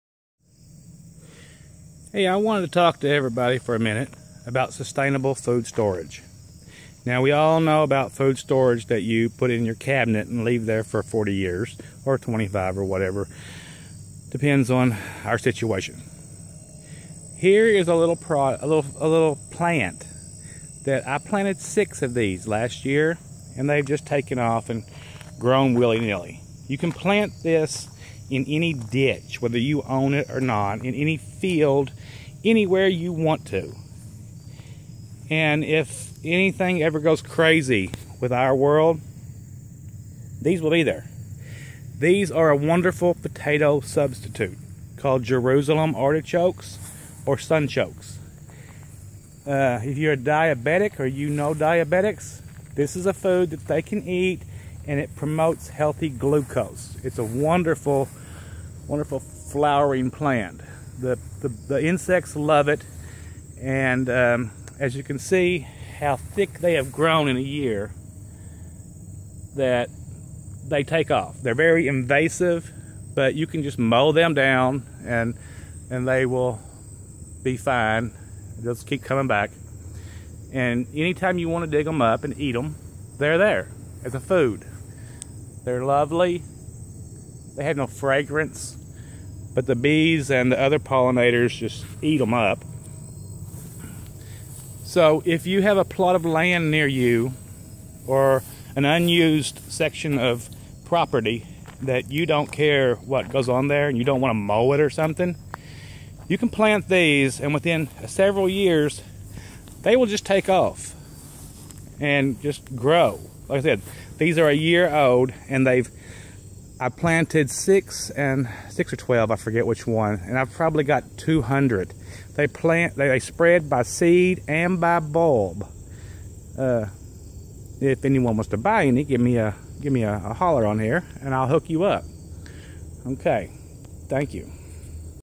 # 36 Sustainable food source. sunchokes ,jerusalem artichoke
Hide food in plane sight. A good source of iron, potassium and thiamin, sunchokes are low in calories and high in fiber. The primary carbohydrate they contain is inulin, which has little effect on blood sugar and is therefore beneficial for diabetes or pre-diabetes.
Jerusalem artichoke is a bumpy, fleshy, root vegetable of sunflower family plants. Its underground nutty, flavorful, starch-rich root is eaten much the same way like potato in many parts of Western Europe and Mediterranean regions.

It should not be confused to globe artichoke, which is an edible flower bud. Similarly, their name is widely misunderstood as “artichokes from Jerusalem” misinterpreted for the Italian girasole carciofi, translating to sunflower artichoke in English. Some of the common names are sunroot, sunchoke, topinambur etc. Scientific name: Helianthus tuberosus.

Jerusalem artichokes are native to the Central America. The plant is technically an evergreen perennial but cultivated as an annual crop. Once established, it grows vigorously with multiple branches, reaching about 5-10 feet height, slightly taller than sunflower plant, and carries many golden-yellow flower heads at the terminal end of branches.

The plant bears many starchy edible rhizomes, firmly attached to stem underneath the soil surface. Jerusalem tubers feature gray, purple, or pink skin externally, and sweet, delicate textured ice-white flesh inside. Some roots have quite bumpy and extremely knobby outer surface, making their clean a tougher task. Each tuber weighs about 75 to 200 g.